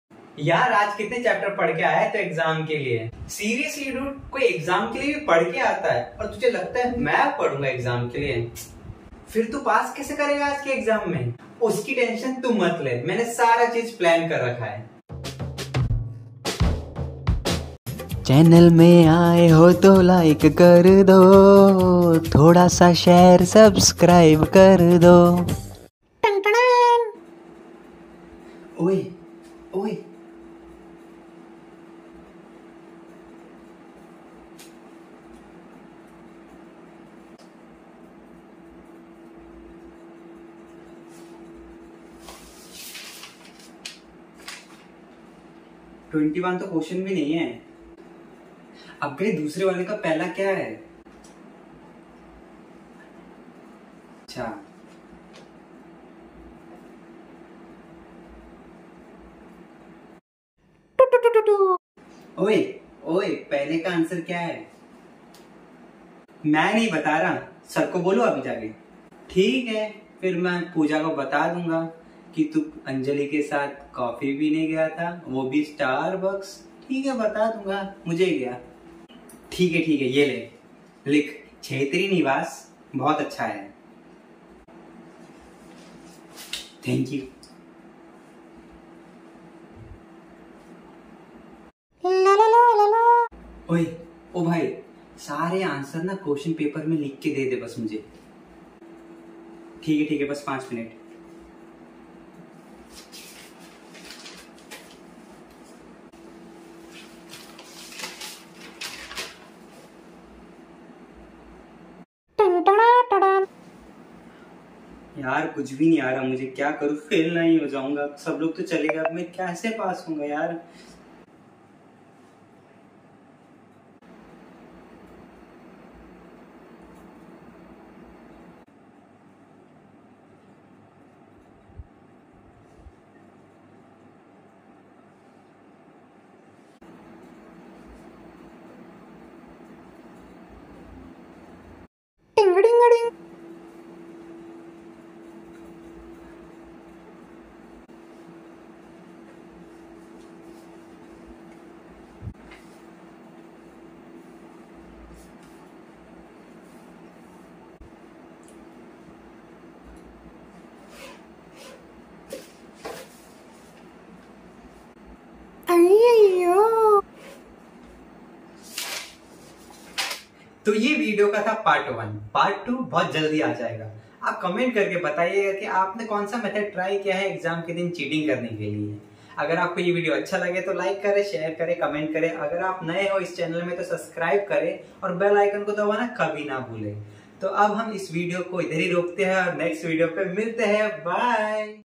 Types of cheaters in exam ft. Indian students| PART-1
hello family kaise aap sab log..I hope aap sabhi acche honge.. this video is all about Types of cheaters in exam 🤣